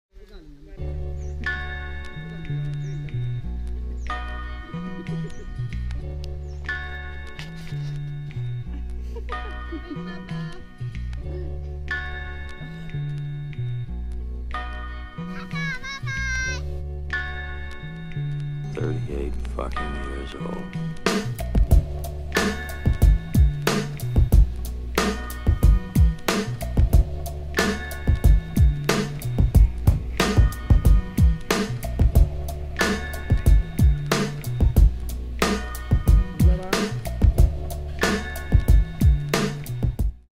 Living with Buddhist monks - Solo Travel Myanmar
Hiking from Kalaw to Inle lake, i stayed with Buddhist monks at their monastery. Beautiful to watch the scenery walking through and quite incredible to see how buddhist live.

19/28